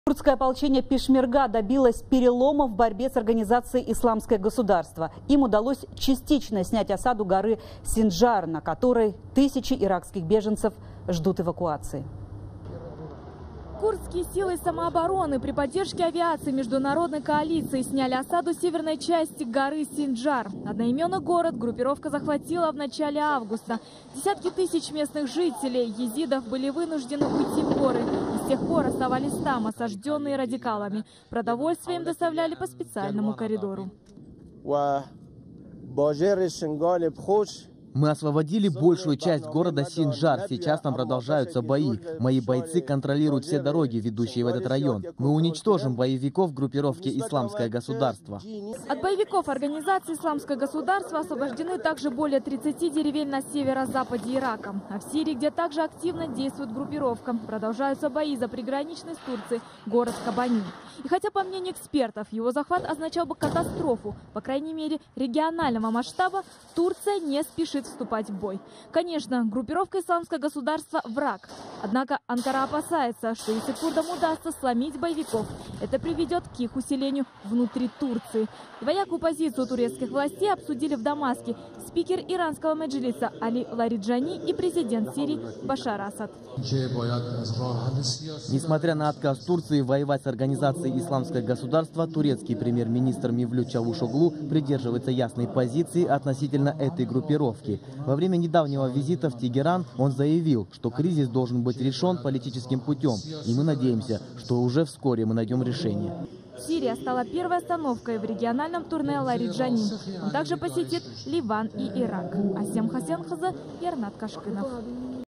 Курды освободили от боевиков ИГ город Синджар
Курдское ополчение пешмерга  добилось перелома в борьбе с организацией  «Исламское государство».